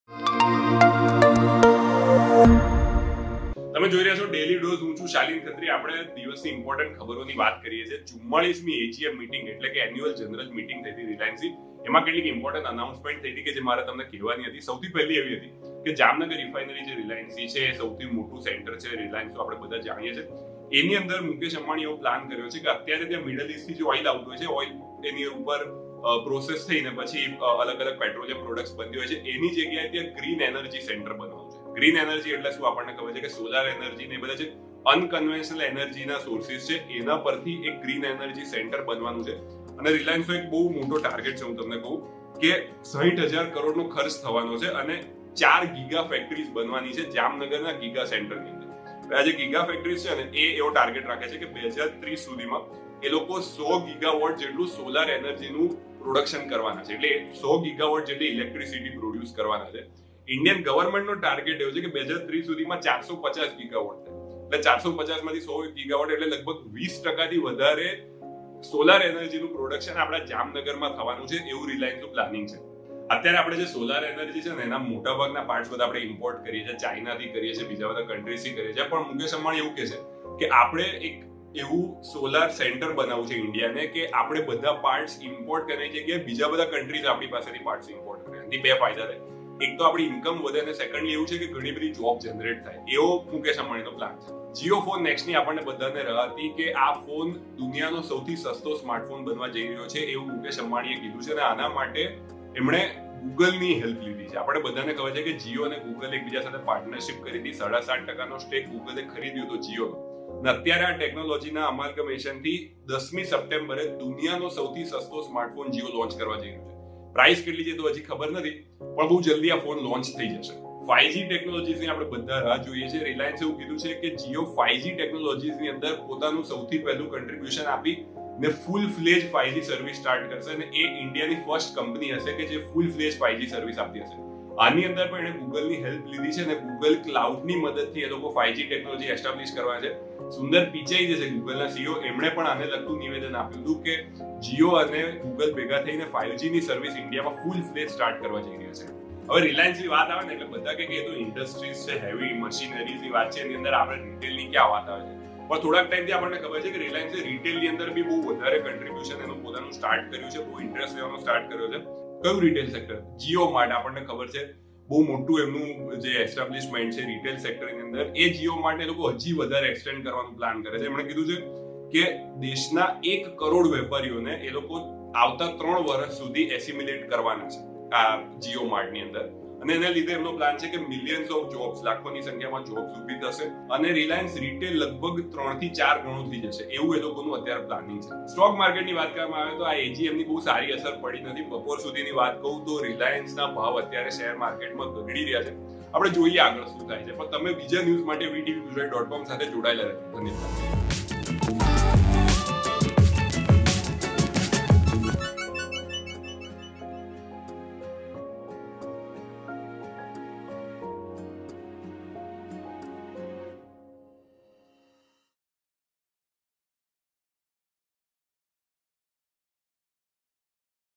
Reliance AGMની આ જાહેરાતો તમારા કામનીસસ્તા સ્માર્ટફોન, જામનગરની કરી વાત | VTV Gujarati | Daily Dose
Reliance AGMની આ જાહેરાતો તમારા કામનીસસ્તા સ્માર્ટફોન, જામનગરની કરી વાત